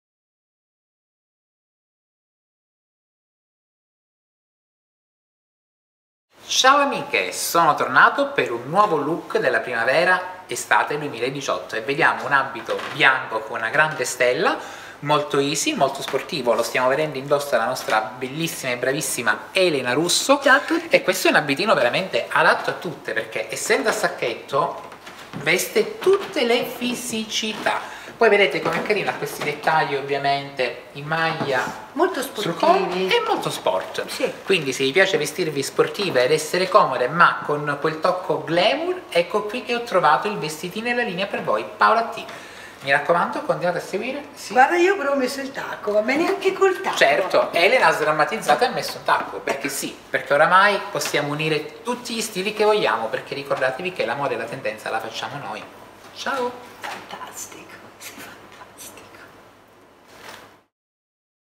I look delle Star : Vestiti anche tu come Elena Russo
Tutte voi potete vestire come le star, basta seguire i miei consigli e i miei look. outfit se anche per le star pensati su misura per voi a prescindere dalla vostra taglia.